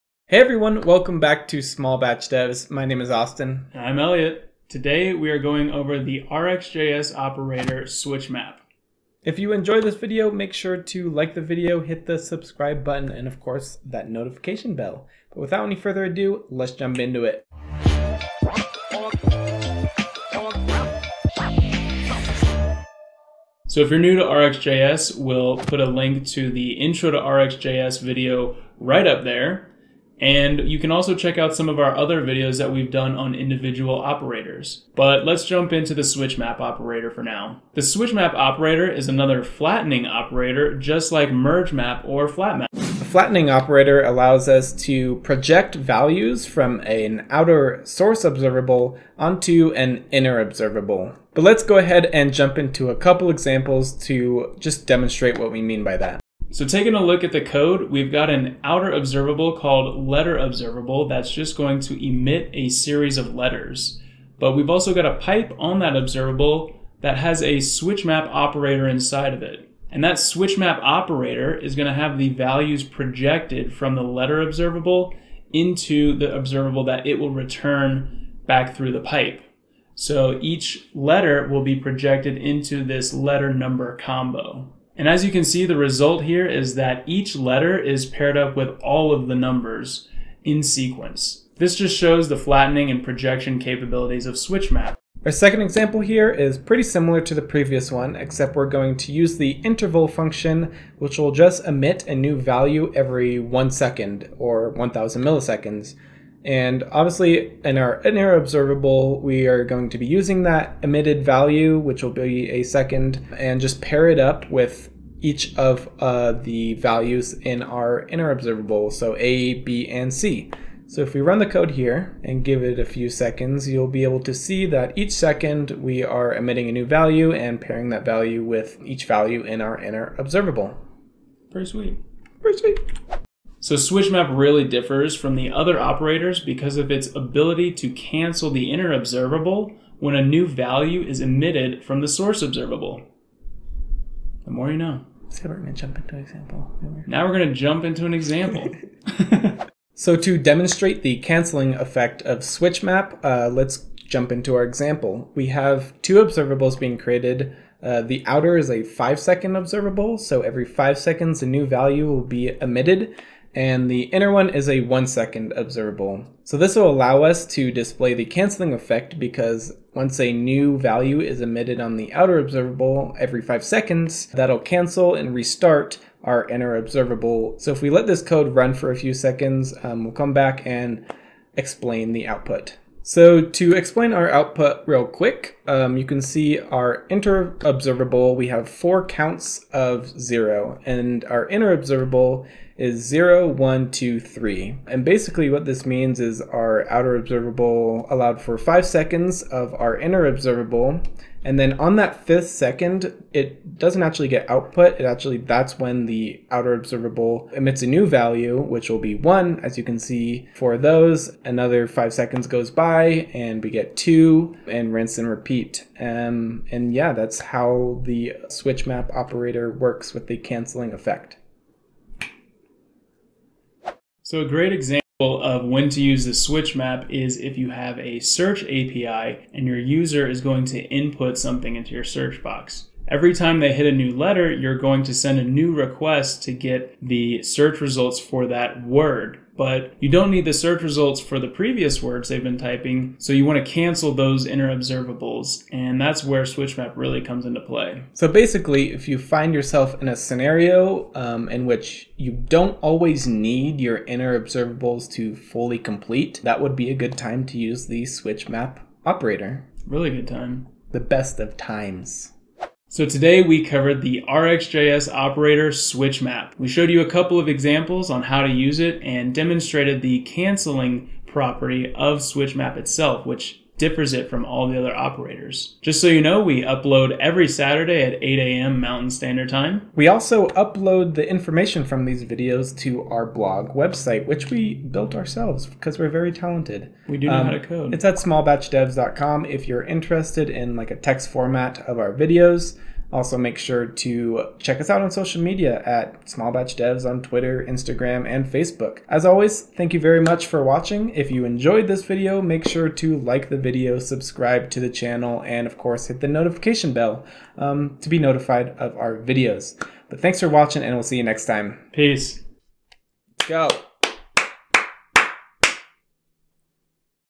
How to Use RxJs SwitchMap with Demos! | RxJs SwitchMap Operator
In this episode, we cover how to use RxJs SwitchMap! It's a very powerful RxJs operator and switchmap is one we use very often! We also show you how RxJs switchmap works with demos and it's awesome observable canceling ability.

KEEP UP with @smallbatchdevs on SOCIAL MEDIA!

00:00 Intro